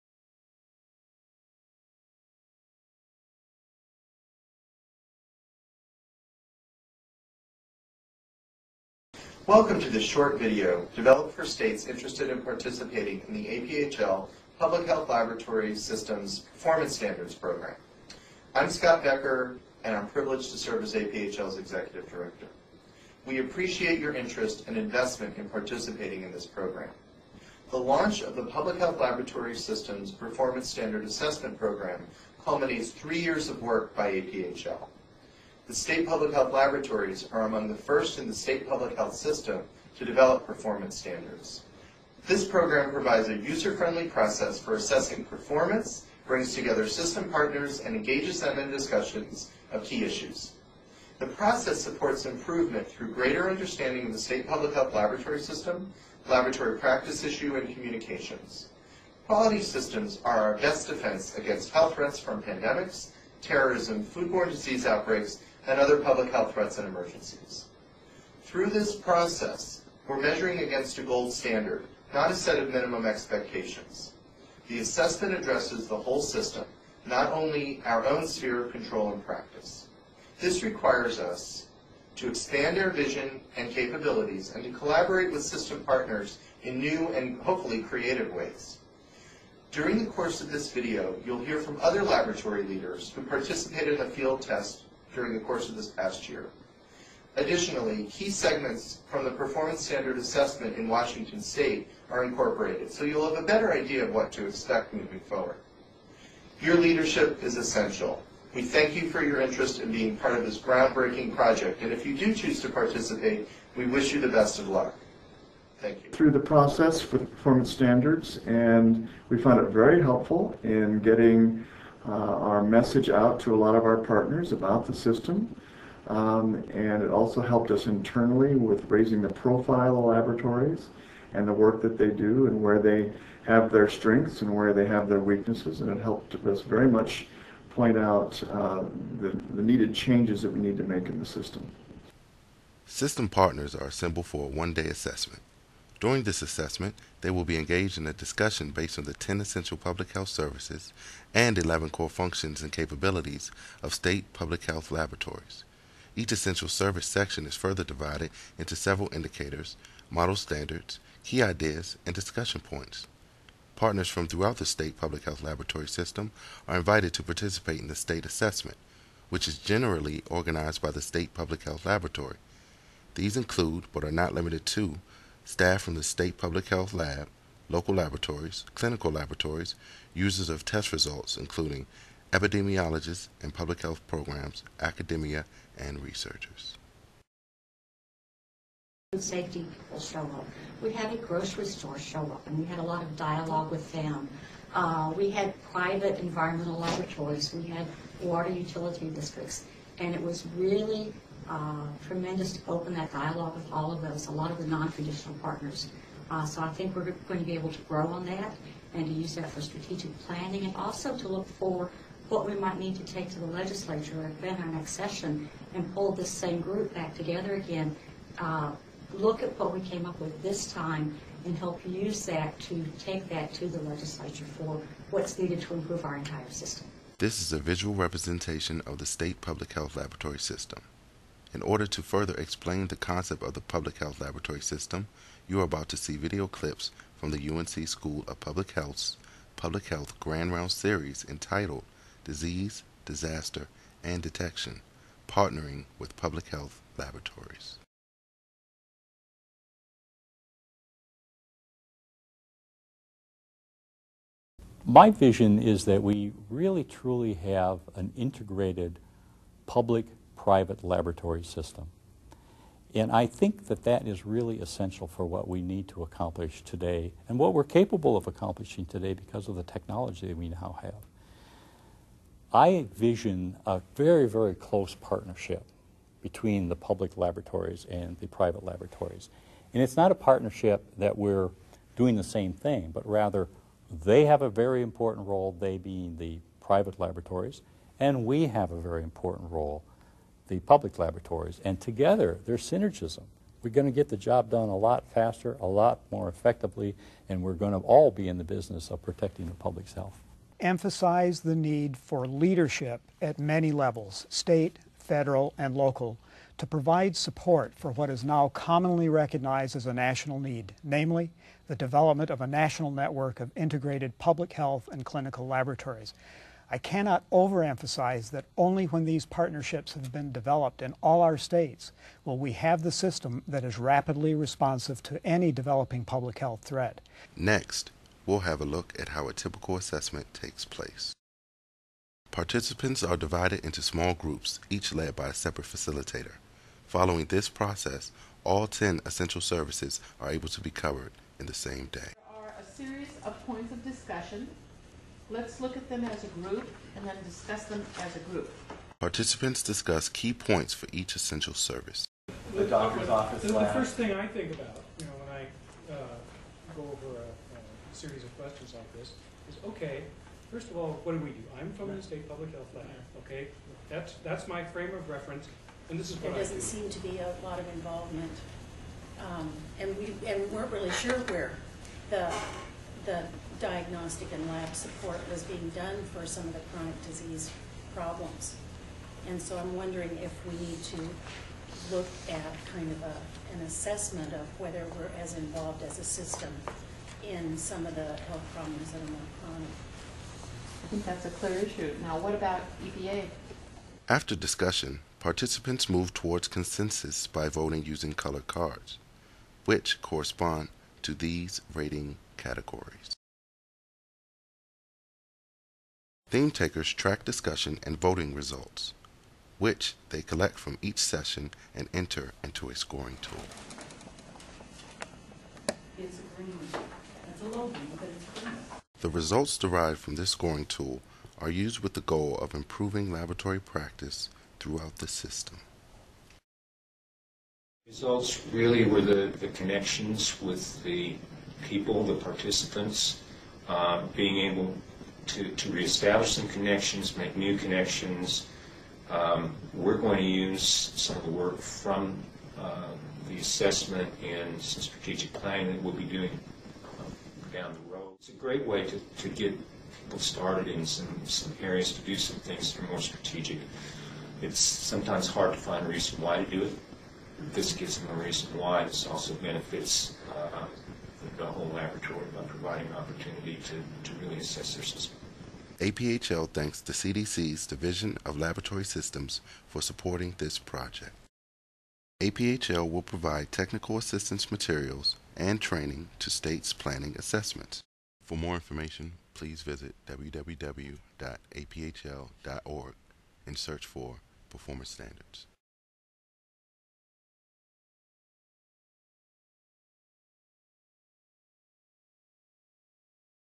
APHL LSIP Program: Laboratory Performance Standards Assesment Video
This video depicts the process of conducting a State Public Health Laboratory Standards assessment.  Most of the footage is taken from the lab assessment at the Washington State Public Health Laboratory.